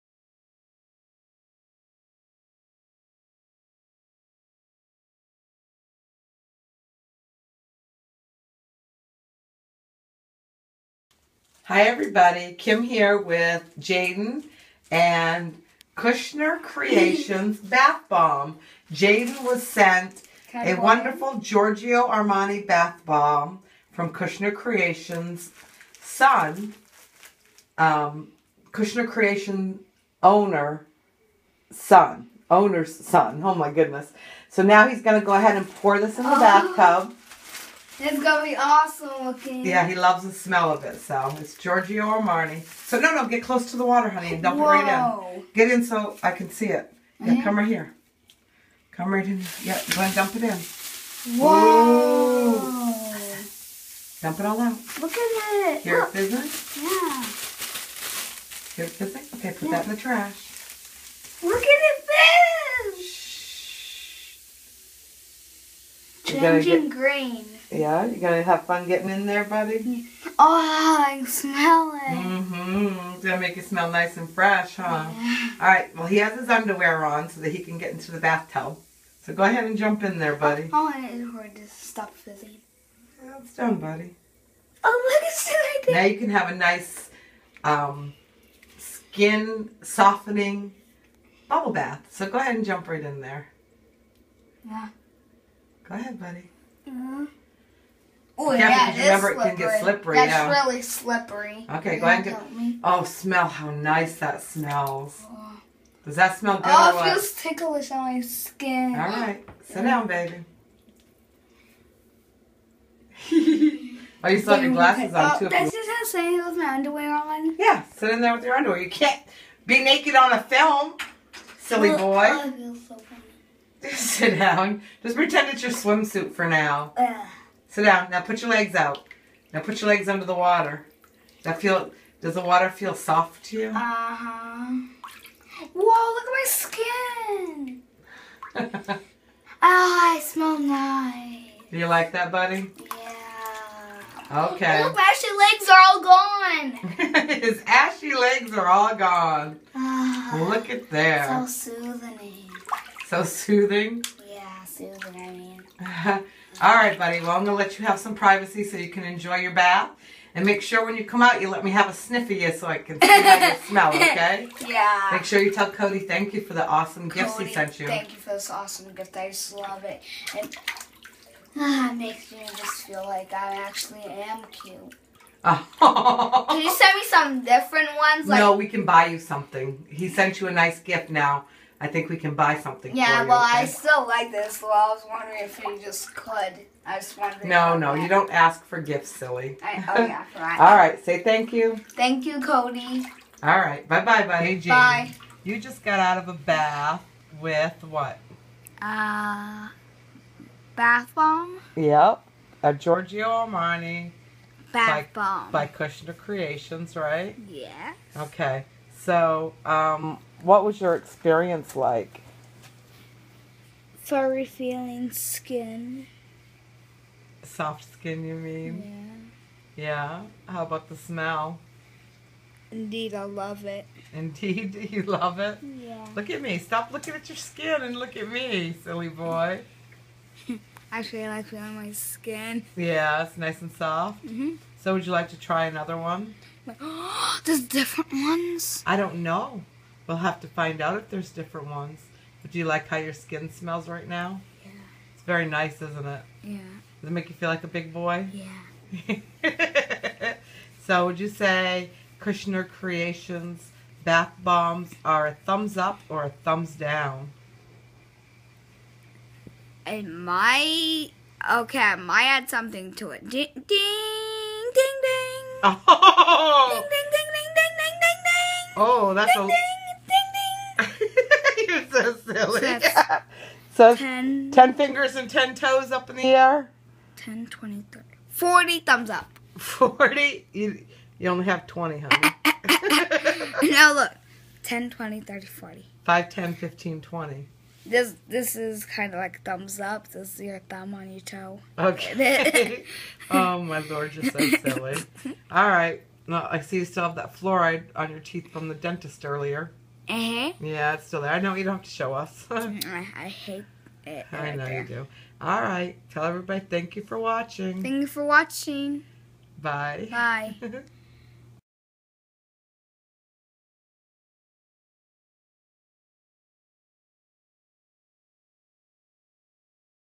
Kushner Kreations Bath Bomb - Giorgio Armani Code Type
Jayden (7 y/o) received a goody bag from Kody (8 y/o), son of the owner of Kushner Kreations. It included a bath bomb, lip balm and 3 Dum Dum Lollipops... which are peanut free. My son and the owner's son both have a severe peanut allergy. Jayden loved this bath bomb and says some of the cutest things when he gets to try it out! Listen closely! You know we ALL love to be pampered and this is what a 7 y/o thinks... so you know the adults would love to be pampered with this as well! Forgive the boxers... we couldn't find the swim trunks... they are put away somewhere! :)